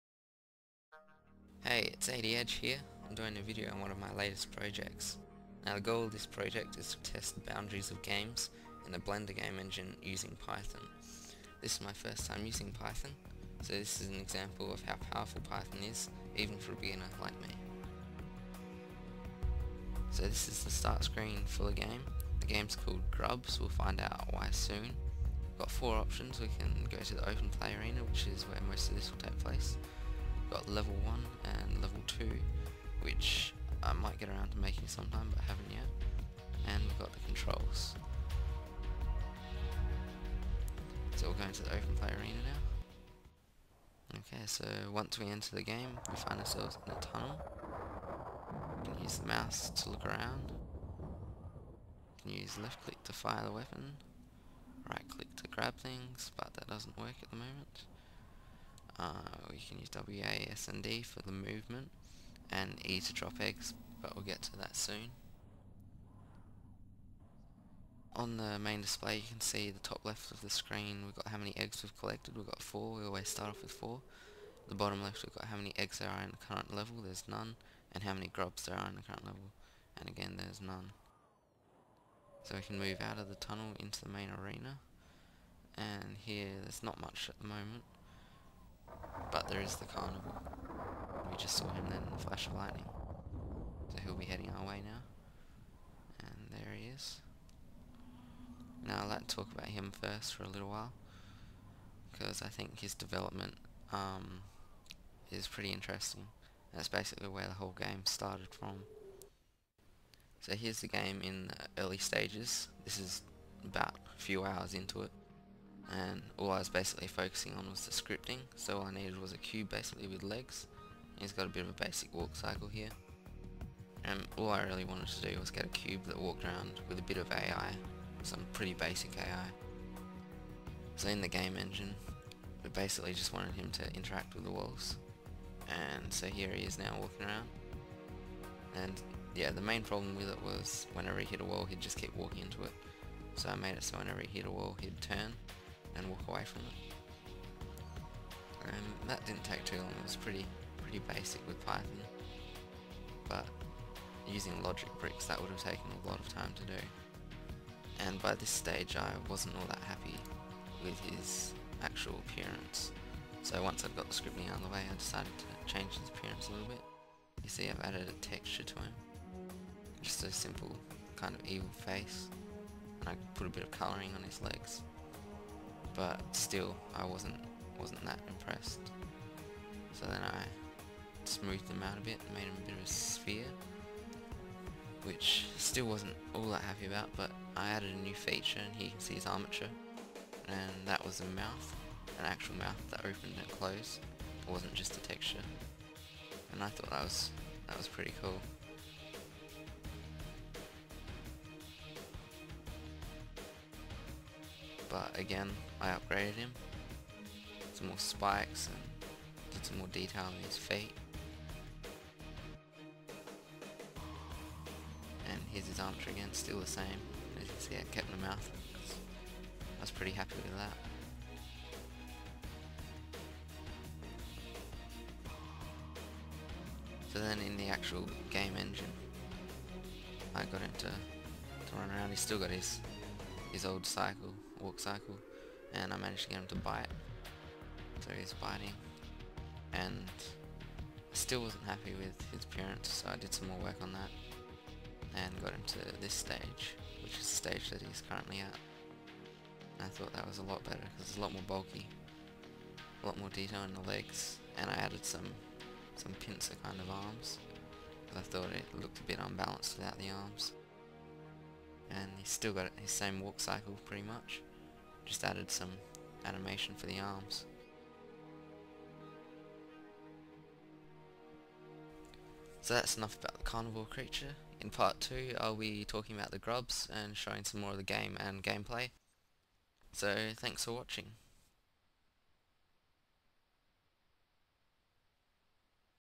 Grubs (Python Game) Pt 1 - Introduction & The Carnivore
The first in several videos about a work in progress game. The game is the first I've ever used python in, so the main goal is to develop and learn from this.

In this first part I'll introduce the Game itself and the creature which started it all - The Carnivore.

Pt 2 (The Grubs) -  coming soon.

And get updates on when I post new videos.
Thanks -- ADEdge